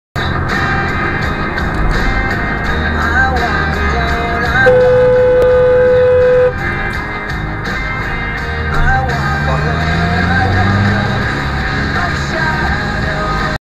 Close call with impatient SUV
With no one behind me, this woman thought it was in her best interest to turn right in front of me.

I'll never understand the stupidity of some people.